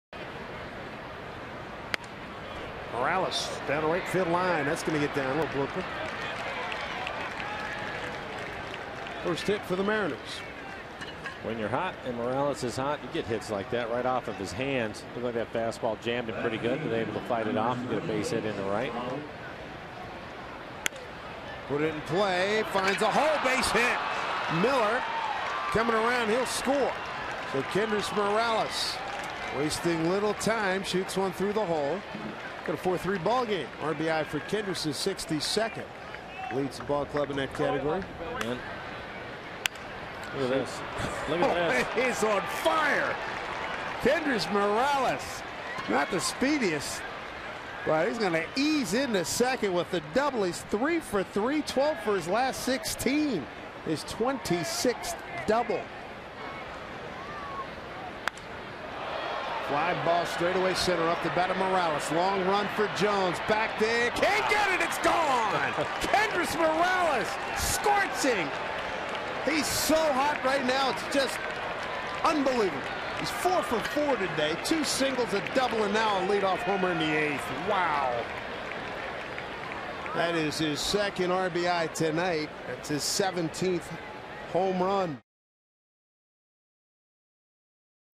SEA@BAL: Morales locked in, goes 4-for-4 with a homer
8/2/13: Kendrys Morales has an impressive night at the plate, going a perfect 4-for-4 with two RBIs while also hitting a homer